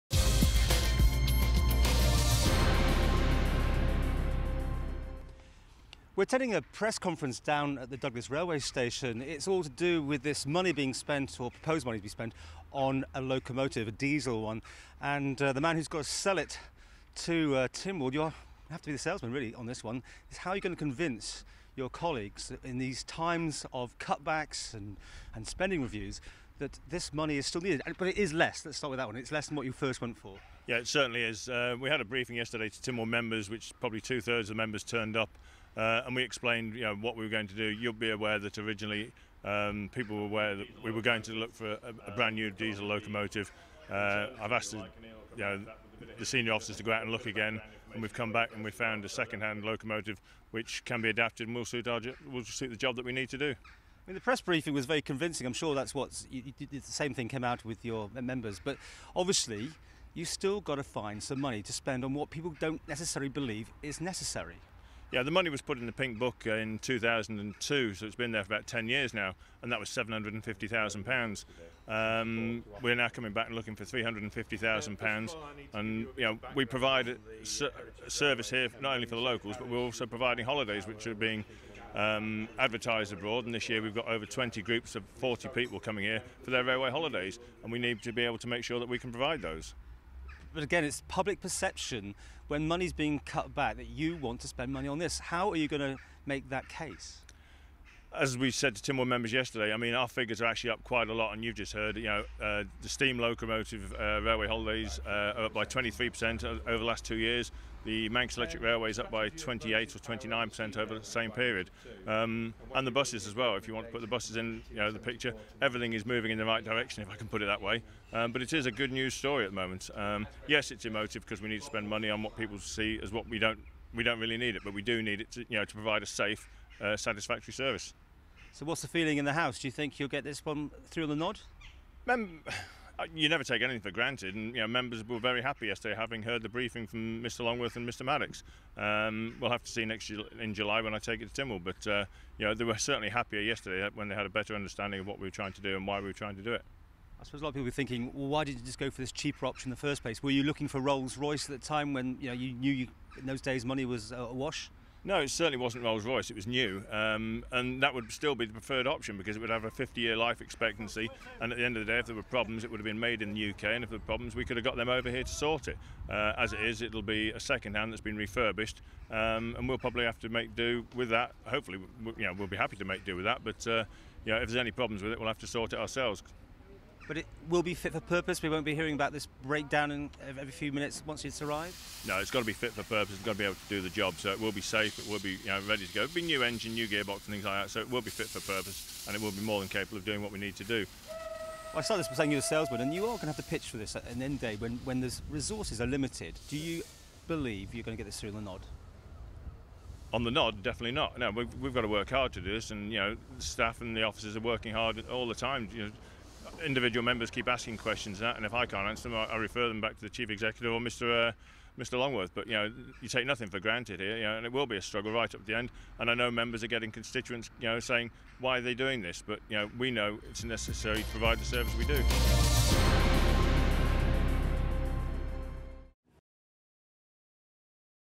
MTTV archive: Diesel Train update (1) 20.6.2012
The Department of Community, Culture & Leisure confirm they are pressing ahead with the proposal for a new diesel train